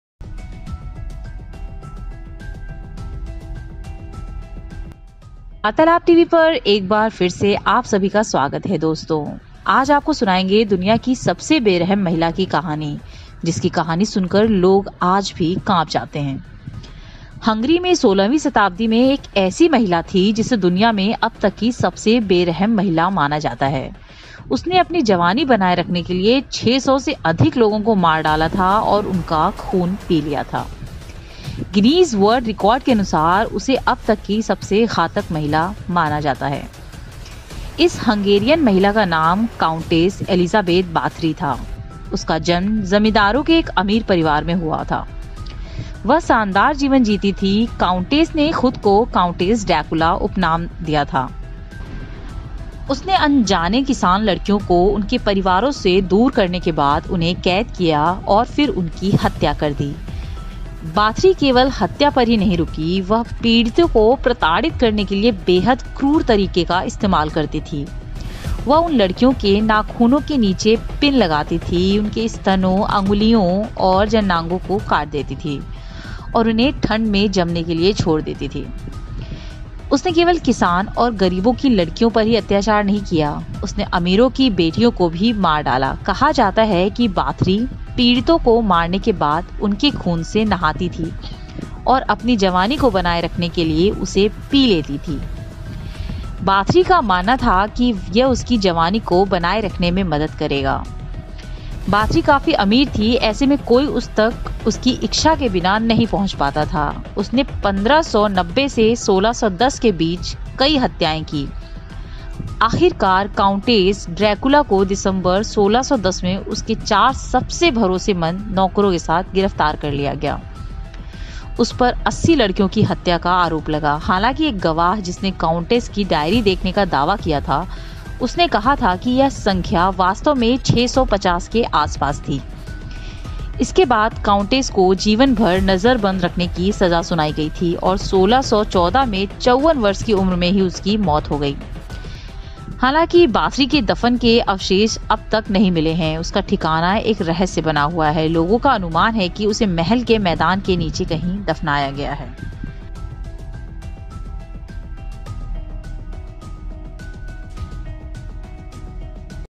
दुनिया की सबसे बेरहम महिला! खूबसूरत दिखने के लिए पीया 650 लोगों का खून
हंगरी में 16वीं शताब्दी में एक ऐसी महिला थी, जिसे दुनिया में अब तक की सबसे बेरहम महिला माना जाता है. उसने अपनी जवानी बनाए रखने के लिए 600 से अधिक लोगों को मार डाला था और उनका खून पी लिया था. गिनीज वर्ल्ड रिकॉर्ड के अनुसार, उसे अब तक की सबसे घातक महिला हत्यारा माना गया है.